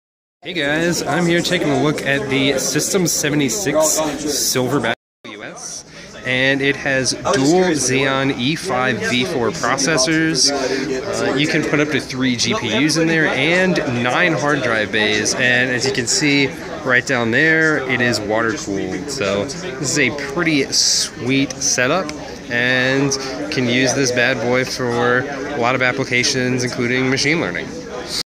Hands on with System76 Silverback WS
Here's an inside look at the System76 Silverback WS server. It sports dual Xeon CPUs, 9x HDD bays, up to 3 GPUs, and as high as 1 TB of ECC RAM.